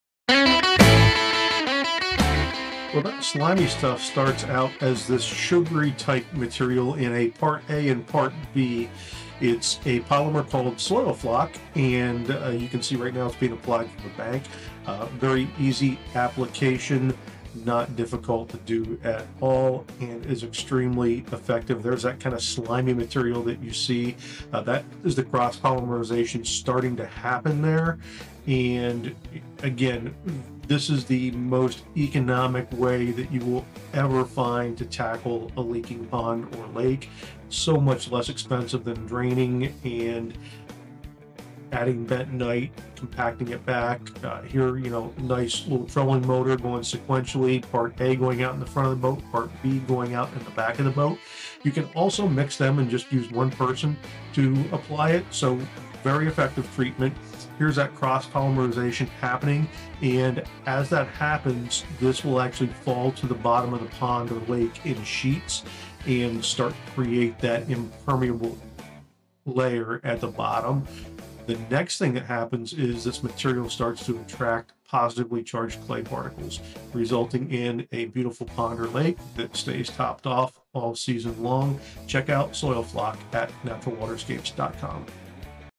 Soilfloc - The most economical leak reduction method for ponds and lakes. Easy to apply.
How to Apply Soilfloc Pond Sealant - Stop Pond Leaks Without Draining

Is your pond or lake losing water? This video demonstrates the simple application process for Soilfloc, the most cost-effective solution for sealing pond leaks without the expense and hassle of draining your water body.

Soilfloc is a two-part polymer sealant that has helped thousands of pond owners and contractors successfully reduce water loss, with over 85% of users reporting significant improvements. This environmentally friendly, non-toxic solution works through an innovative dual-action process that makes it remarkably effective for both widespread seepage and localized leaks.

Watch the complete application process for this professional-grade pond sealant that requires minimal labor and equipment. The polymer crystals are simply broadcast over the water surface, where they sink and are naturally drawn to leak areas by water flow. Part A and Part B work together to create a powerful sealing system that continues working long after application.

**How Soilfloc Works:**

The two-part polymer system combines linear and cross-linked polyacrylamides that perform two critical functions. First, the polymers swell to over 300 times their weight when they contact water, physically plugging cracks and crevices in your pond bottom. Second, the anionic polymers attract and bind positively charged clay particles, creating a self-reinforcing seal that becomes part of your pond's soil matrix over time.

This cross-polymerization layer doesn't just provide an immediate fix – it continues attracting fine particles to strengthen the seal, making it a long-lasting solution. The polymers follow the natural water flow directly to problem areas, ensuring the sealant goes exactly where it's needed most.

**Perfect For:**
- Farm ponds and irrigation reservoirs experiencing water loss
- Golf course water features and hazards
- Natural ponds and small lakes
- Koi ponds with earthen bottoms
- Livestock watering holes
- Any water body with soil seepage issues

**Why Choose Soilfloc:**

Unlike other sealing methods that require complete dewatering, heavy equipment, or expensive contractors, Soilfloc offers a DIY-friendly solution that can be completed in a single day. There's no need for costly pond draining, fish relocation, or reconstruction work. The product has been thoroughly tested in university studies for safety with aquatic organisms, making it suitable for ponds with fish and wildlife.

Application rates are straightforward – each 110-pound unit (55 lbs Part A + 55 lbs Part B) treats approximately 3,000 square feet of pond bottom. The granular formula resembles sugar crystals and can be applied by hand, scoop, or fertilizer spreader from the shore or a small boat.

**Results You Can Expect:**

While some immediate effects may be visible, the full sealing effect typically develops over three weeks as the polymers fully hydrate and attract soil particles. The treatment creates a flexible, self-healing barrier that adapts to minor ground movement and continues to strengthen over time.

Whether you're dealing with widespread seepage through porous soil or significant localized leaks through cracks and crevices, Soilfloc provides a proven, economical solution that saves water, time, and money compared to traditional pond repair methods.

For best results, remove any deep silt accumulation or heavy vegetation before application, and apply during calm weather conditions when the pond is actively leaking – this ensures the polymers are drawn directly into problem areas.